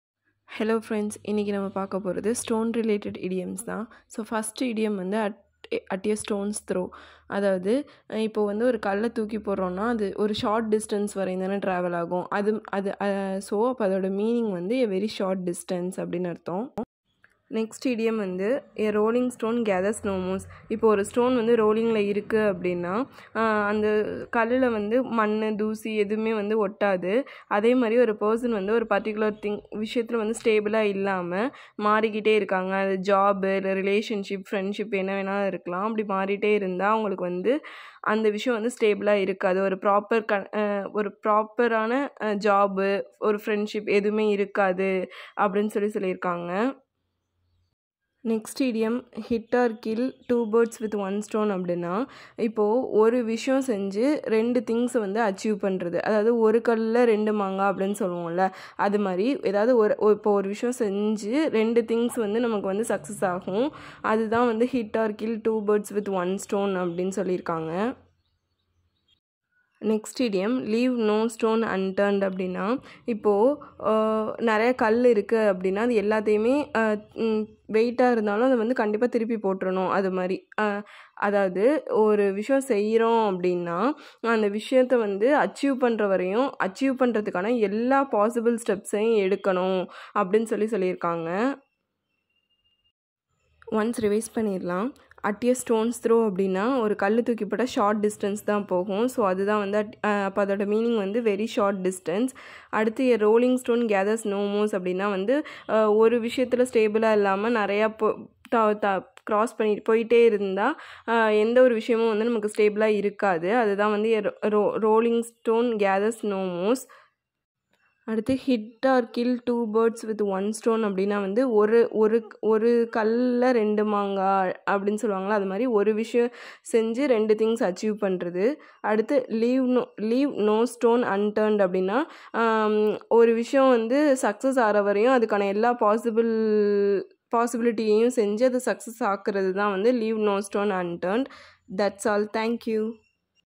Idioms and phrases💥(related to stone)
In this video Idioms related to stones are explained. this will be useful for all and especially for competitive exams.